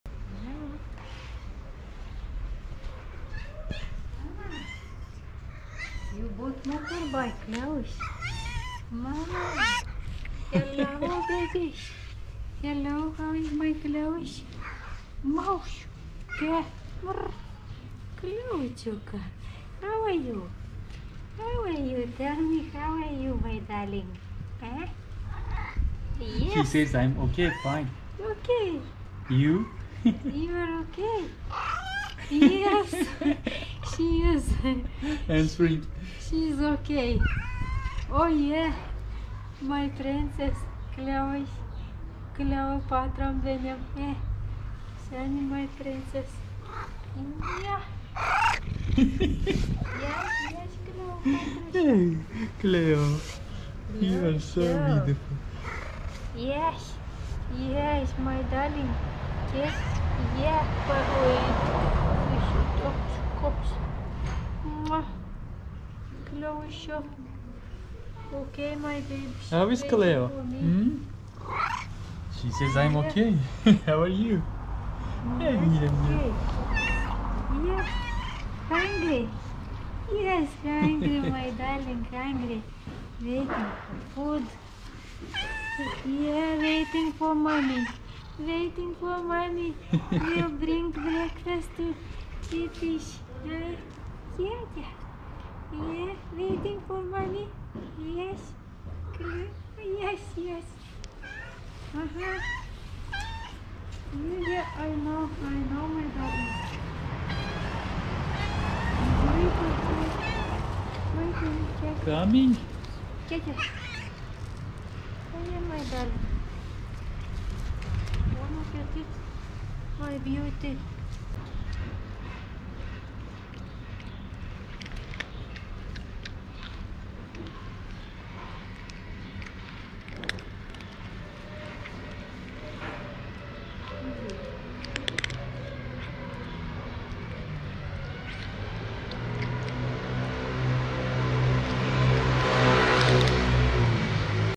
Orange cat talking to me with her unique meow is unbelievably cute
Cleo cat talking to me with her unique meow is unbelievably cute and affectionate.